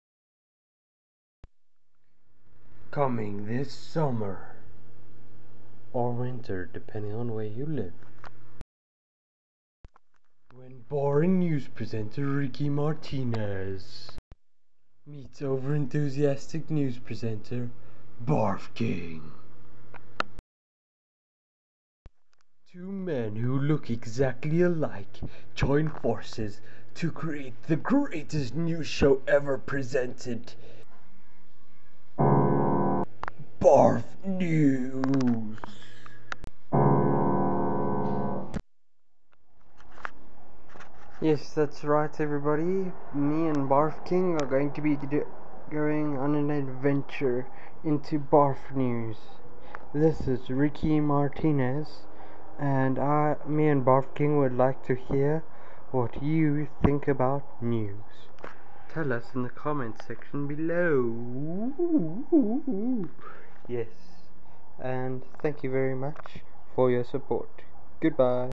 Movie trailer parody-Barf News episode #2
This is our movie trailer parody enjoy.

What do you think of news, what are your favorite news shows? Tell Barf King in the comment section below.

Tell Barf King what you would like to see in the news. He will put it in and also send a shout out to you. Thanks